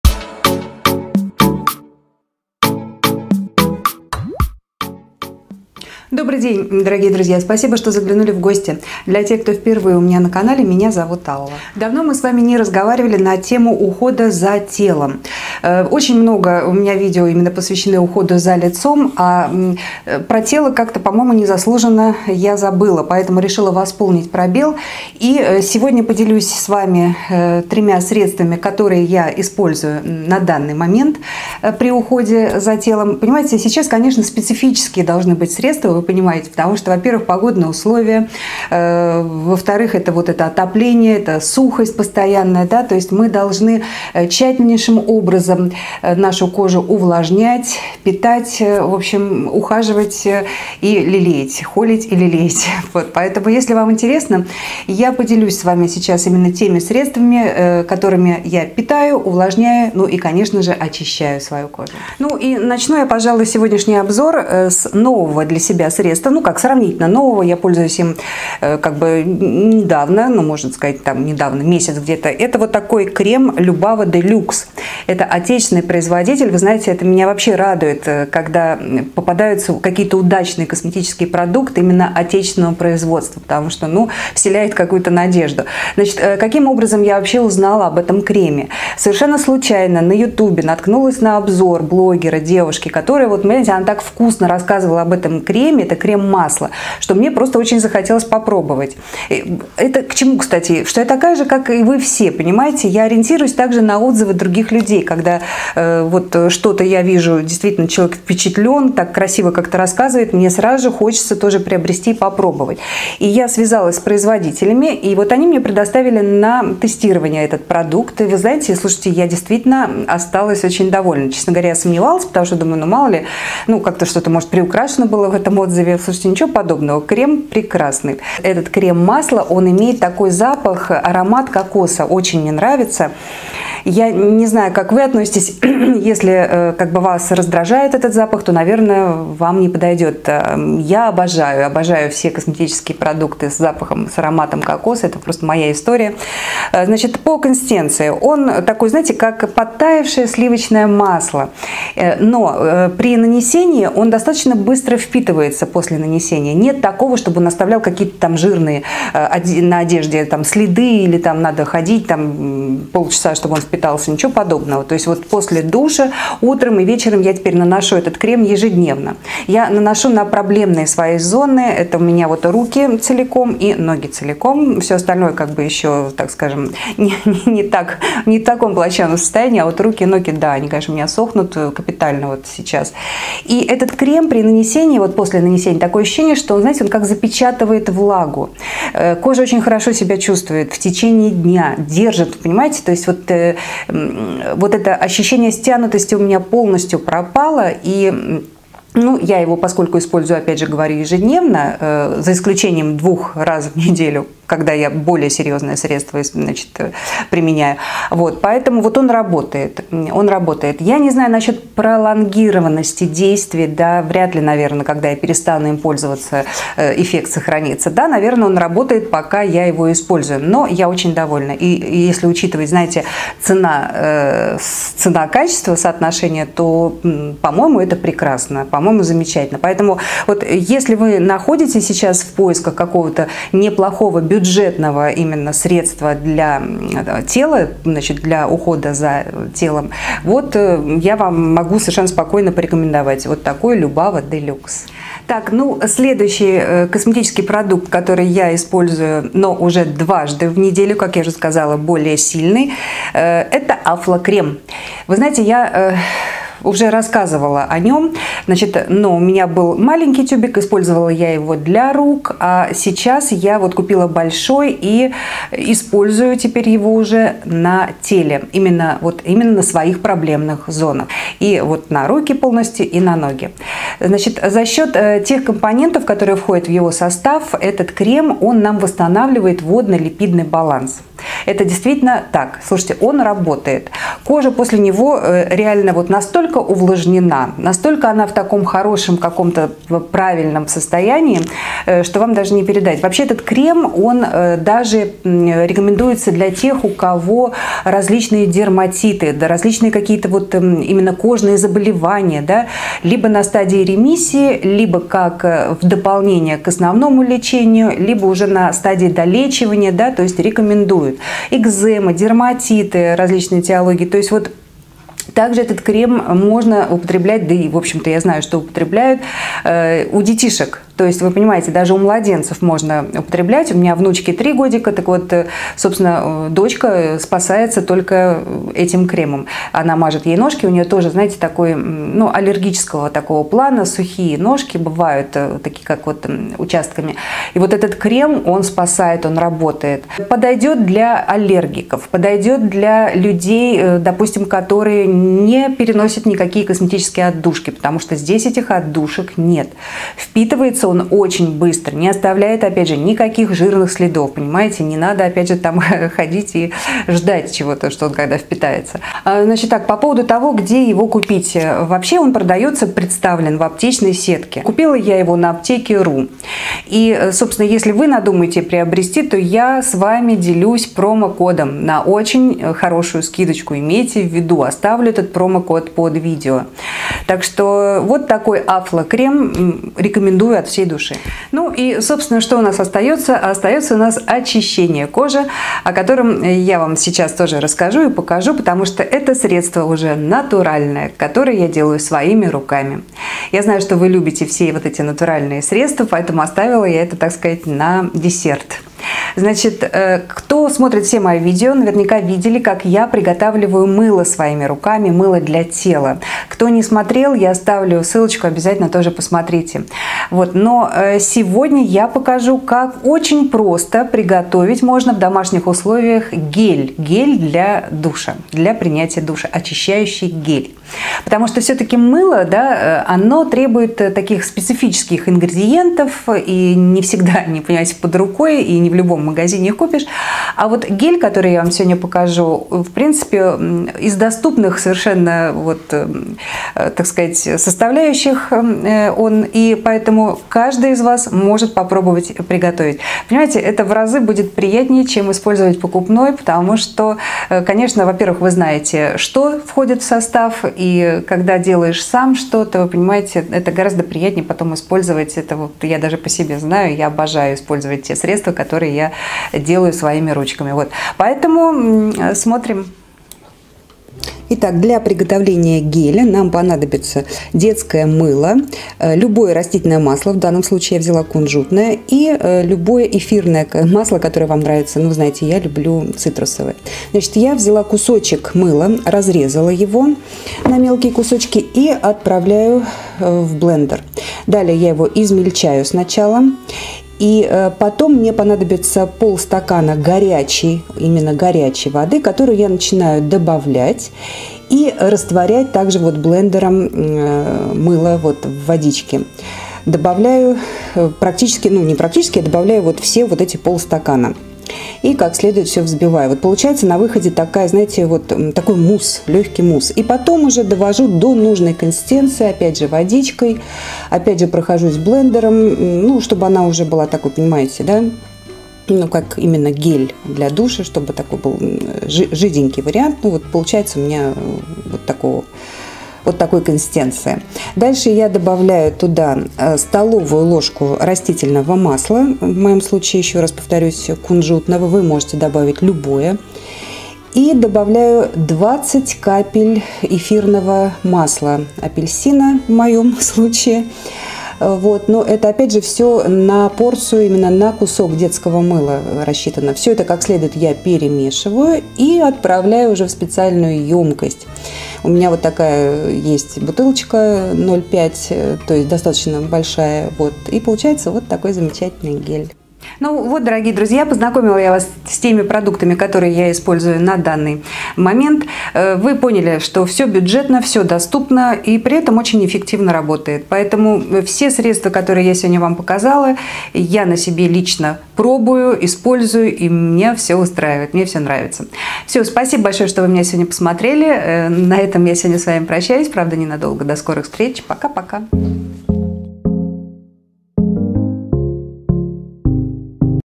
ШЁЛКОВАЯ кожа // НАТУРАЛЬНЫЙ гель СВОИМИ РУКАМИ.
Приветствую Вас!)
С приходом зимы, не только кожа лица, но и тела нуждается в особом уходе. Холодное время года – это испытание для нее. На улице холод и ветер, в квартирах и офисах пересушенный воздух. В результате появляется шелушение и сухость. Чтобы этого не случилось, кожу нужно защищать и питать.

 СРЕДСТВА по уходу из видео:
1. Lubava Deluxe Крем-масло для тела
3. Гель для душа. Рецепт.
Детское мыло                     1 кусок
Масло растительное         1 стл
Эфирное масло                  20 капель
Если Вы захотите меня отблагодарить, то Вы можете на выбор:
2. Написать комментарий под видео.